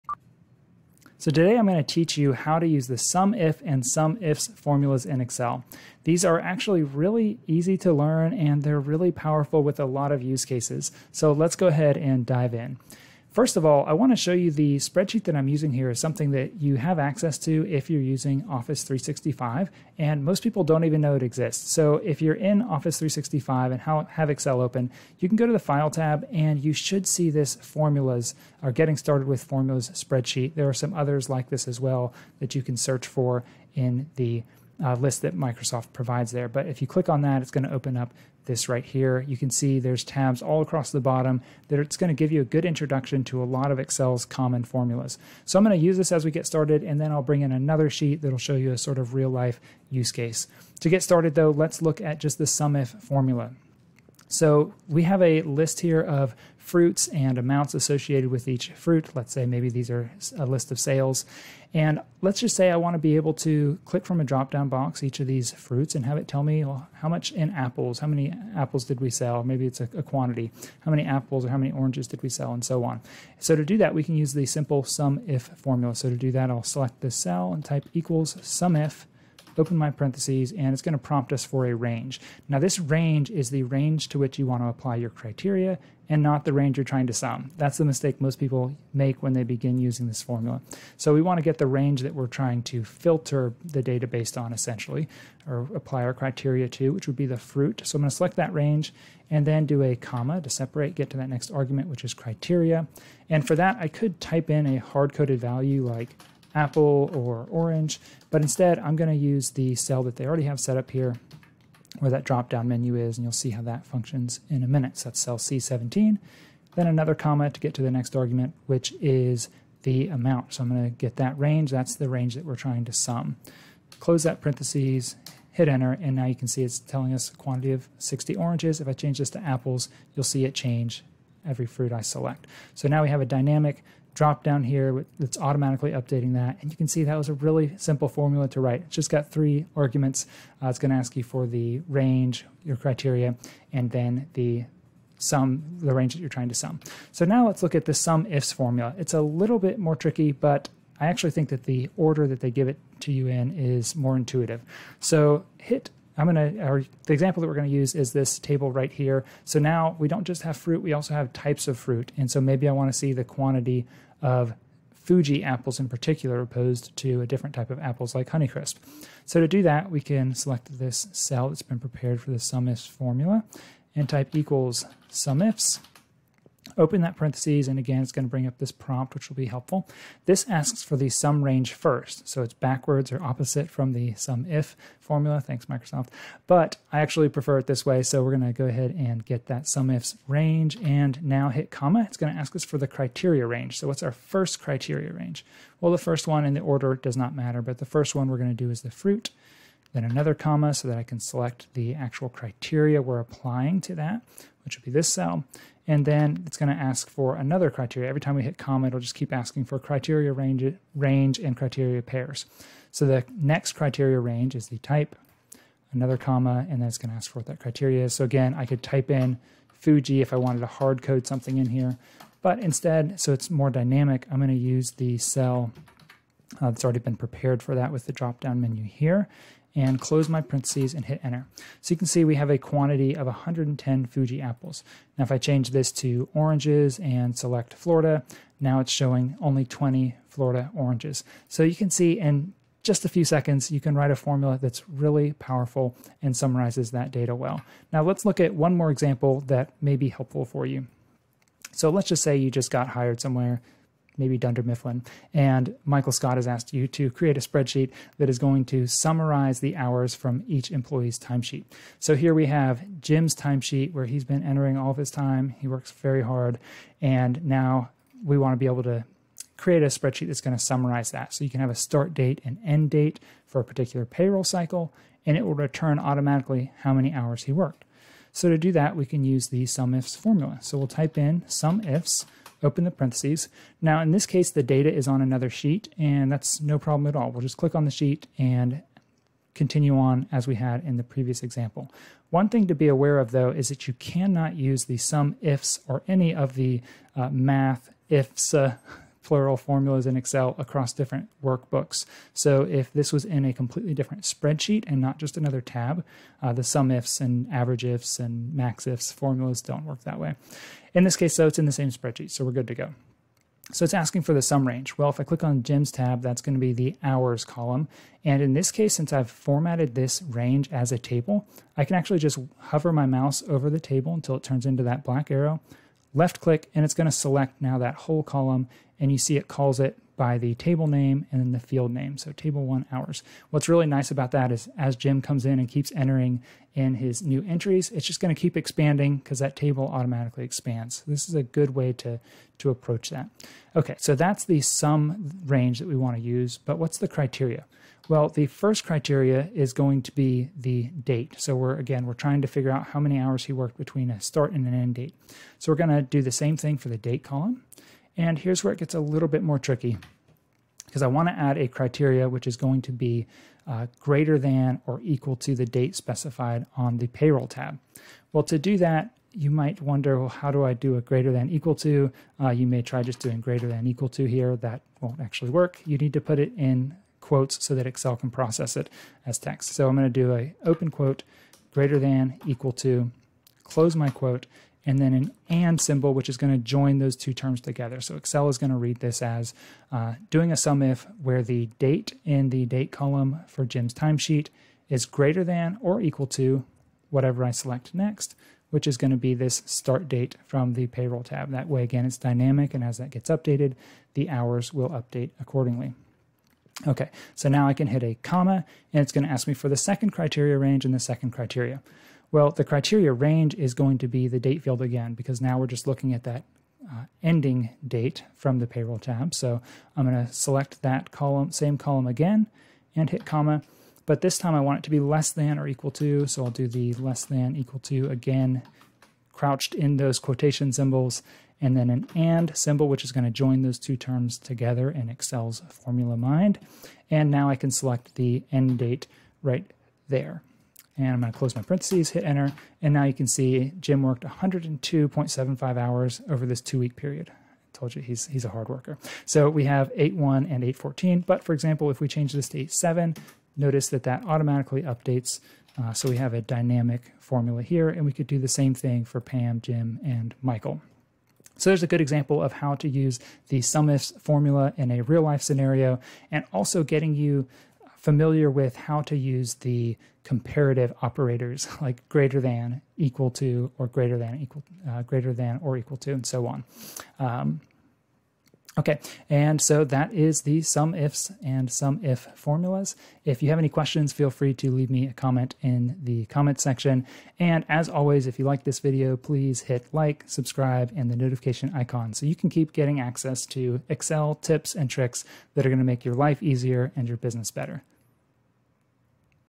Tutorial: How to use SUMIF & SUMIFS formulas in Excel
In this easy-to-follow tutorial, I'll show you exactly how to use the SUMIF and SUMIFS formulas in Microsoft Excel.

These formulas are really easy to learn and are powerful tools for quickly summarizing datasets of any type/size in Excel.